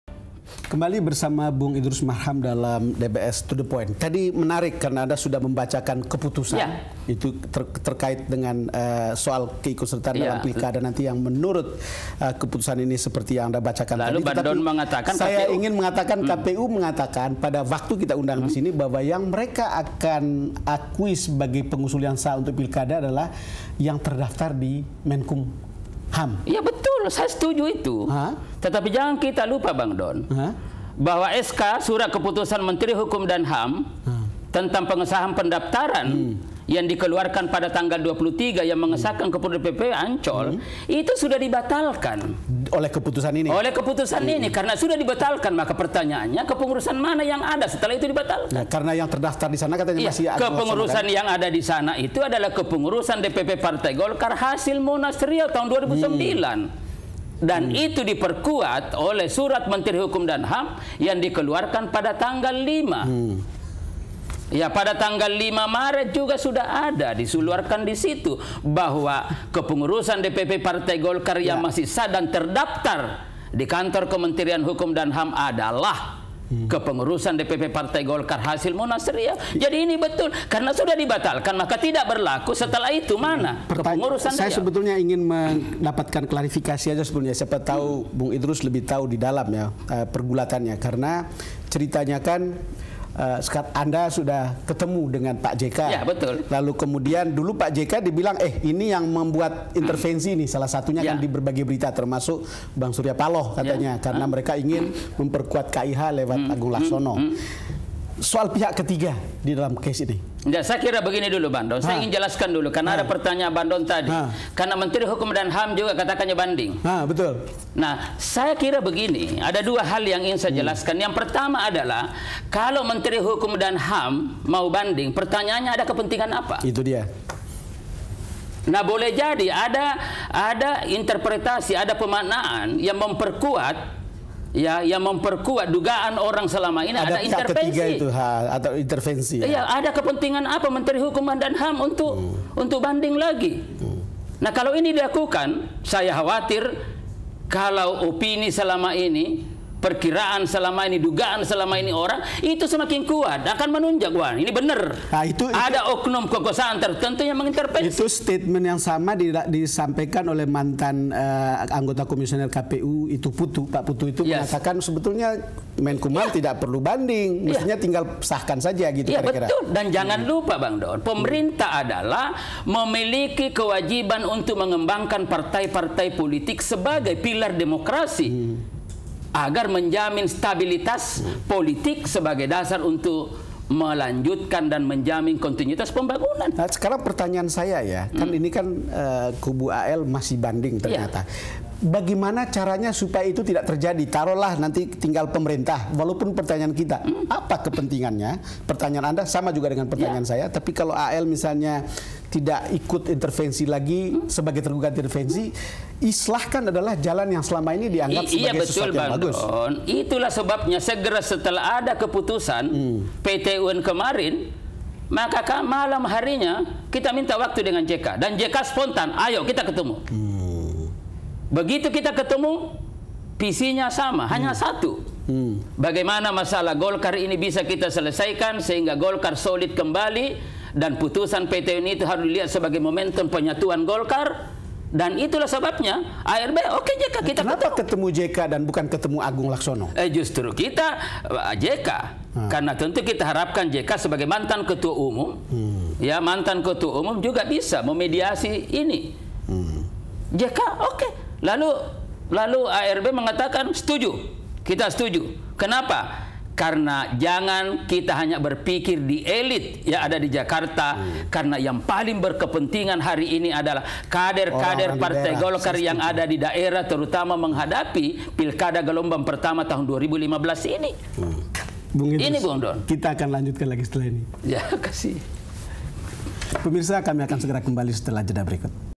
DBS To The Point: Jalan Panjang Konflik Golkar #3
Konflik internal Partai Golkar belum menemukan solusi permanen. Lama tidaknya konflik, tergantung konsistensi kader kepada sistem. "Karena kekuatan Golkar ada pada sistem yang efektif," kata Sekjen Golkar Idrus Marham.

Facebook.com/BeritaSatuTV
Youtube.com/BeritaSatu